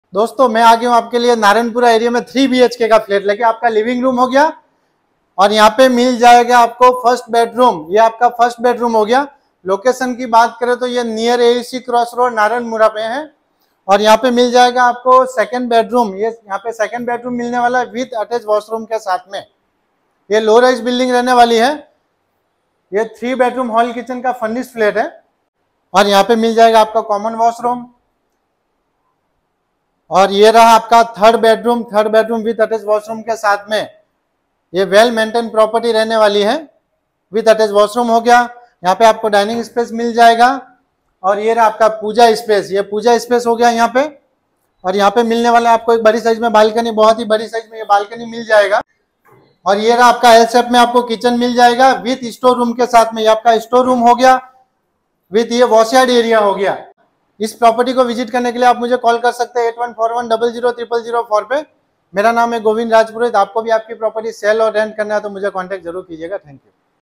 3 BHK Semi Furnish Flat For Sale Near AEC Cross Road Naranpura Ahmedabad M:-8141000004
3 BHK Semi Furnish Flat For Sale Near AEC Cross Road Naranpura Ahmedabad

👉 3 bhk Apartments
👉 Semi Furnish Flat
👉 5 Storey building
👉 Size -165  Sq Yard
👉 16 Years Old Aprox
👉 Possesses Ready

☎️ Call More Details Contact
  Velly Property Expert

Note:- Call Time 10:00 Am To 7:00 Pm

if You Like Our Video Follow @vellypropertyexpert